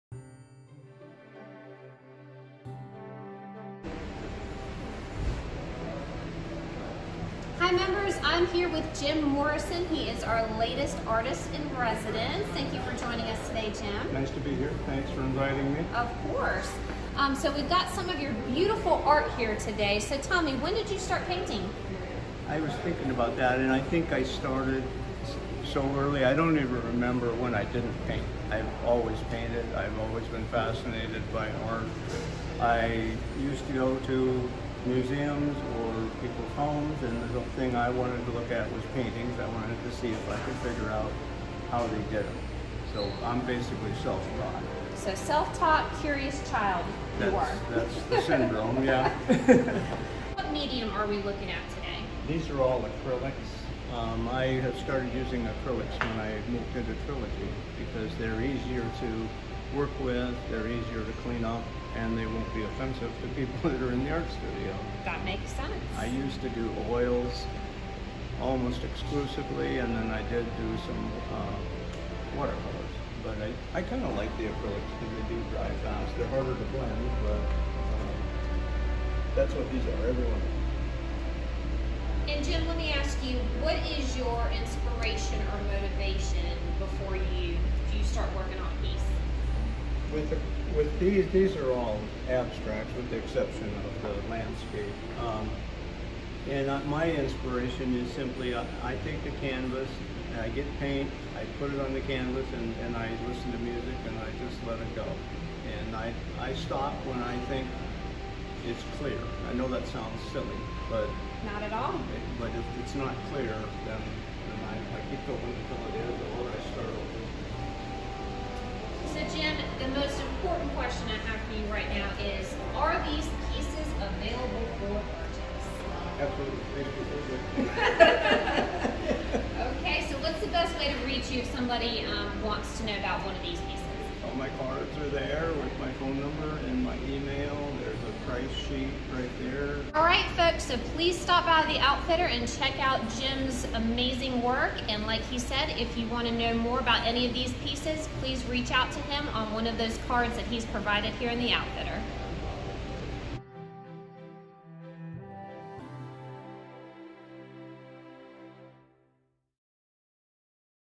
Artist in Residence | Jim Morrison
Jim Morrison has his canvas art featured in the Outfitter.